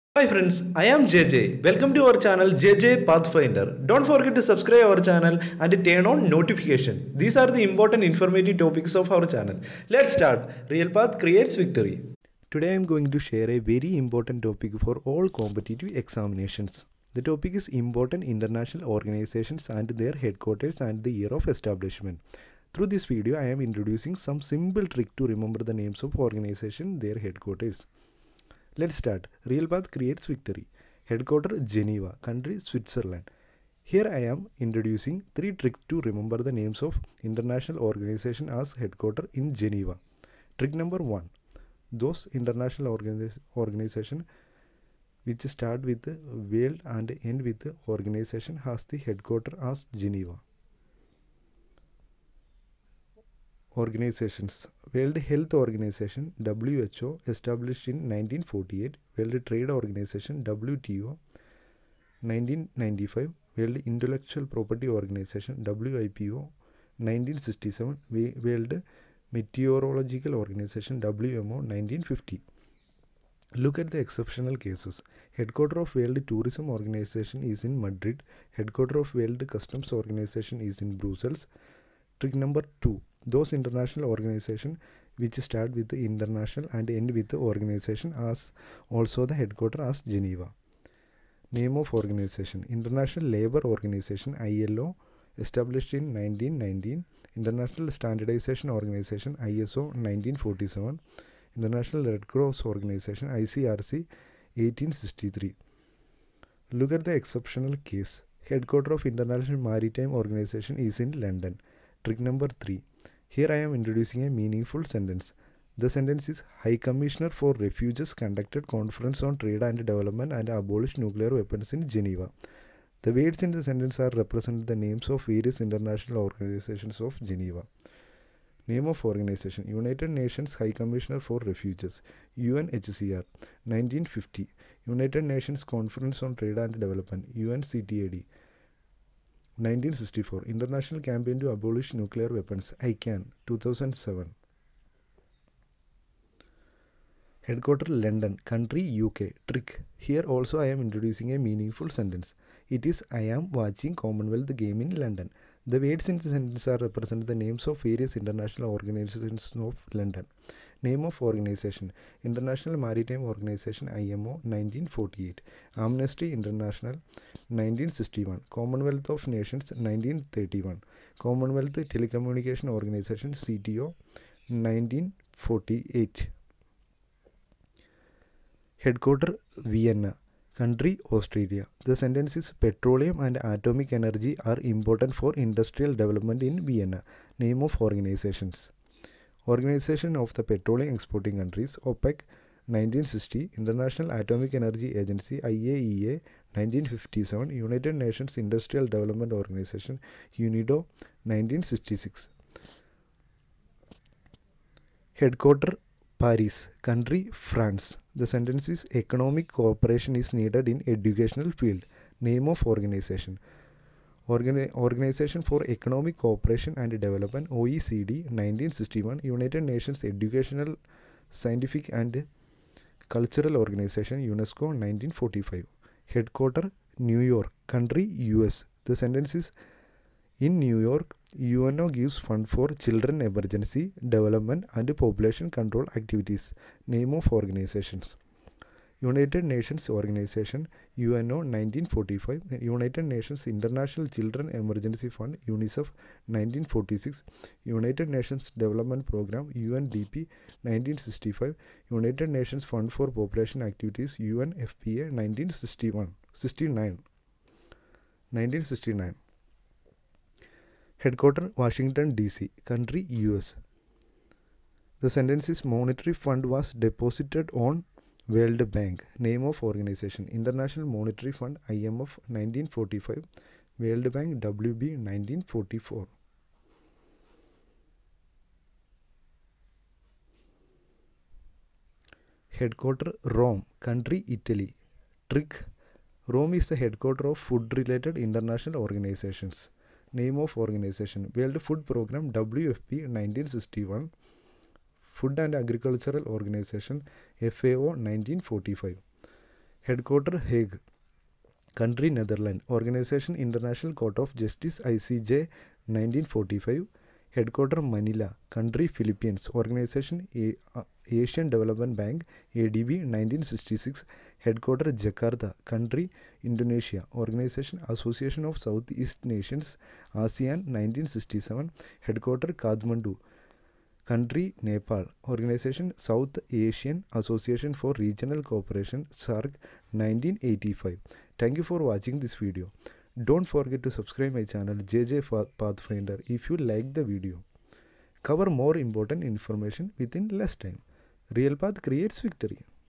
International organisations and their headquarters | Trick to remember 🔥
Important international organisations and their headquarters and the year of establishment.In this video has included some tricks to remember the name of international organisations their headquarters